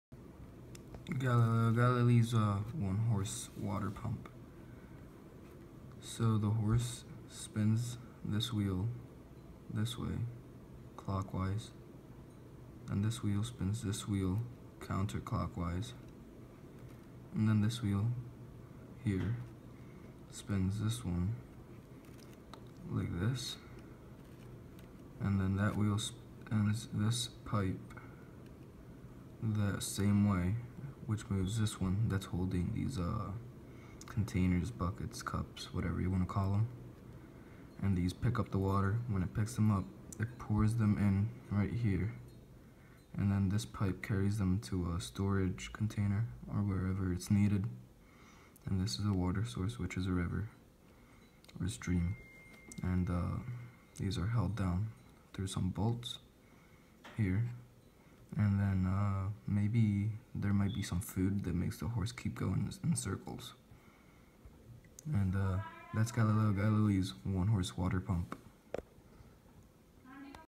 Galileo Galilei's One Horse Water Pump!!
Galileo Galilei's Single Horse Water Pump.  He was born on Feb. 15 1564.  In Florence Italy and he died on January 8 1642 at the age of 77 in Tuscany Italy.